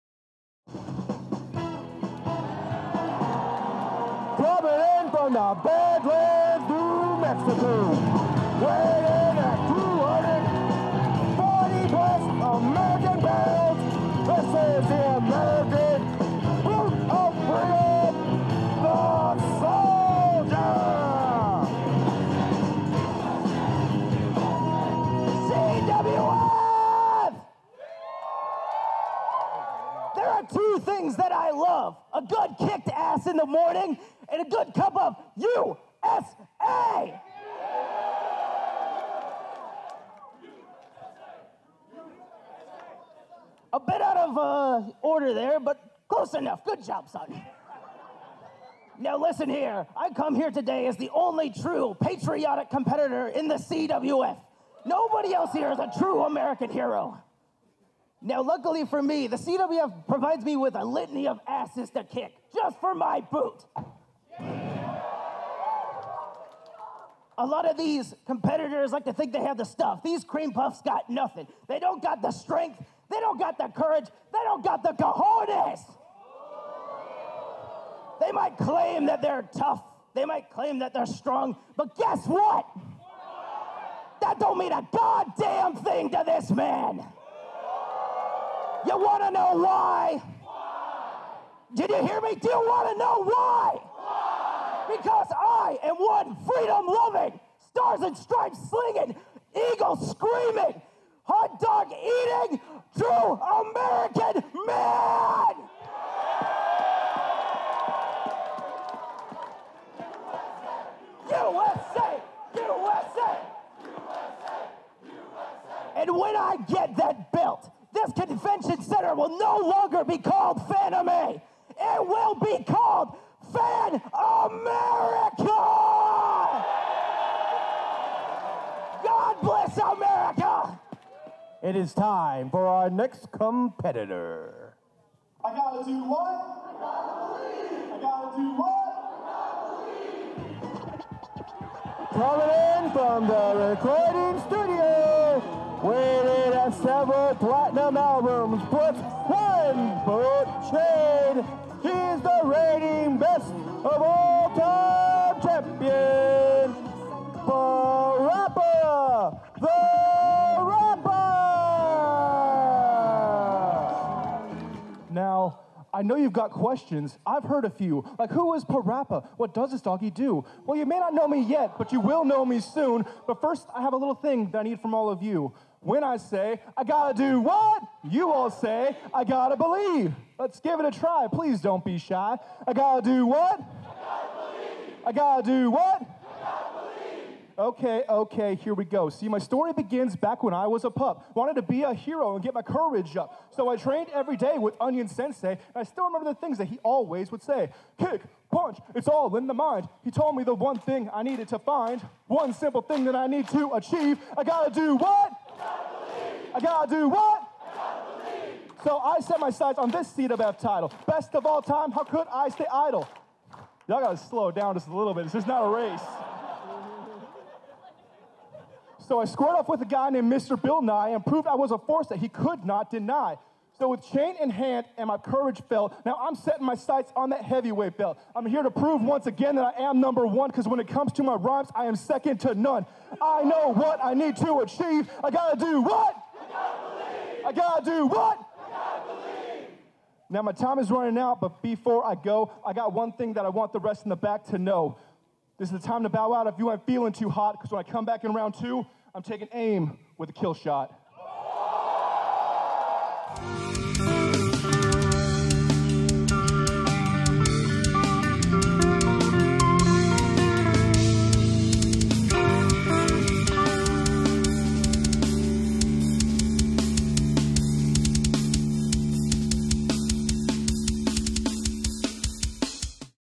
FanimeCon 2024 | Event - CWF: Fanimania 9 - The Soldier/PaRappa the Rapper
The CWF celebrates 10yrs of operating, and this night is Fanimania 9! Let's meet the final two competitors of Round 1!  @CosplayWrestlingFederation
Related Links
Cosplay Wresting Federation